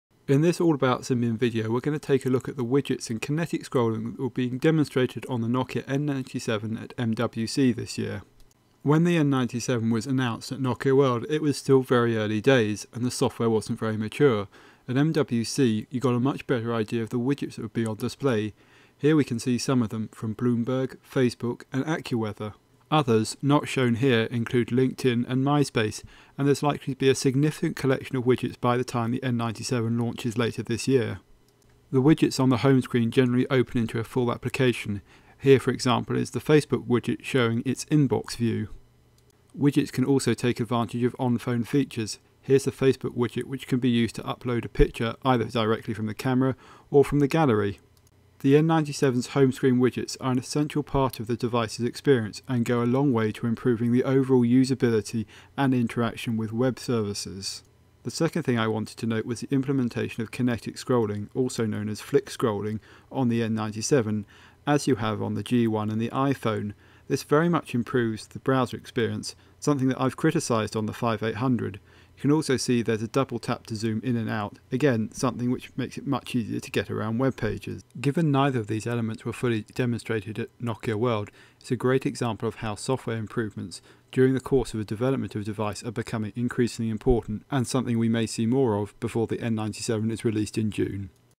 Nokia N97 widgets (Facebook) and kinetic scrolling
Nokia N97 at MWC 2009. Two quick examples of software additions since its launch at Nokia World. Home screen widgets (including a fully implemented Facebook widget) amd kinetic (flick) scrolling in the web browser.